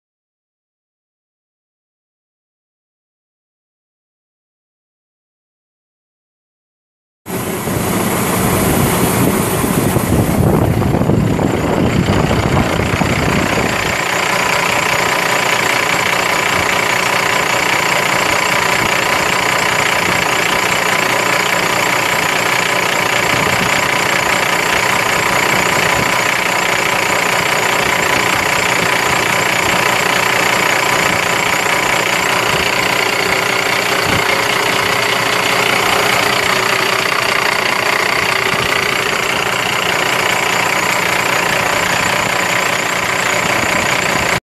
2004 Volvo VNL semi truck for sale | no-reserve Internet auction February 23, 2017
2004 Volvo VNL semi truck
690,147 miles on odometer
Cummins ISX 14.9L L6 turbo diesel engine
Eaton Fuller ten speed automatic transmission
Model RT0-16910C-A52105
Tandem axle
AM/FM/CD
Heated mirrors
Air suspension seat
Block heater
PTO
Wet kit
Mud flaps
Air suspension
Air brakes
11R22.5 tires
GVWR 50,000 lbs
View repairs
Kansas title
4V4NC9TG14N363463
It's located in Montgomery City, MO, and we're selling it to the highest bidder regardless of price.